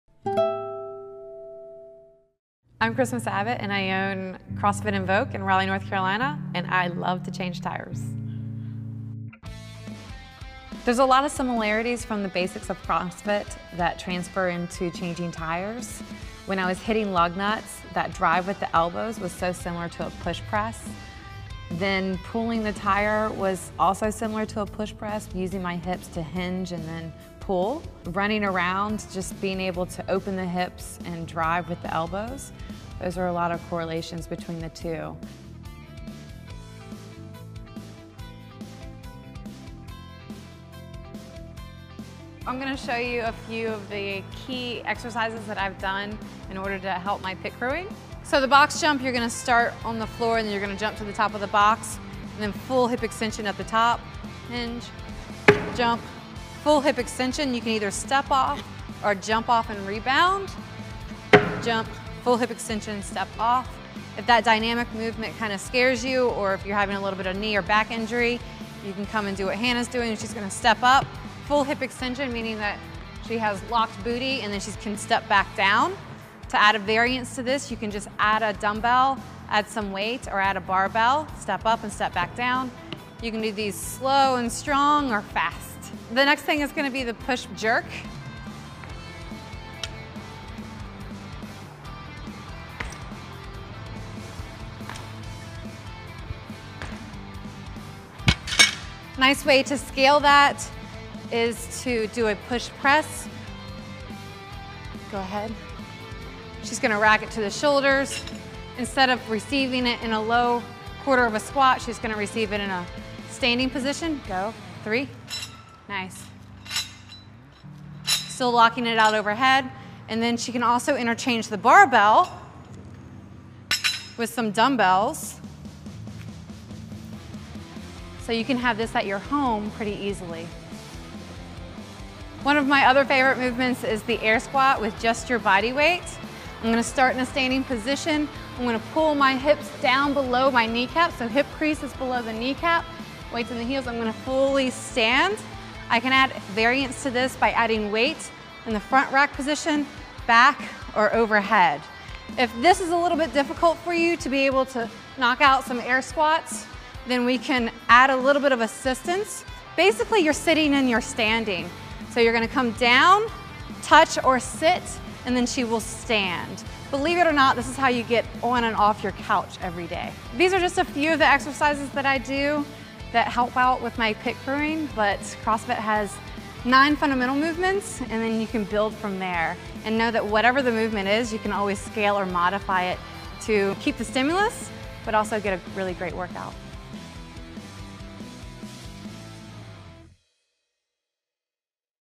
Christmas Abbott teaching Some exercises from CrossFit Invoke
Christmas Abbott explains some of the CrossFit Invoke Exercises which general people can also do at home and keep themselves fit. Not to forget. She is the first woman at NASCAR and is fantastic athelete who will also be competiting in the Olympics this year.. HOPEFULLY !